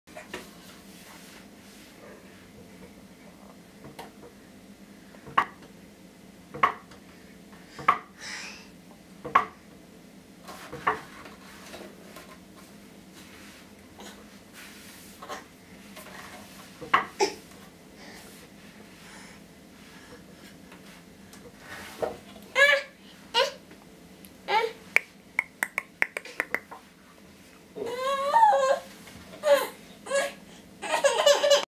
Rachel trying to climb out her rocking horse - 09.05.2015
Rachel trying to climb out her rocking horse
Getting too big now & frustrated to have the sides on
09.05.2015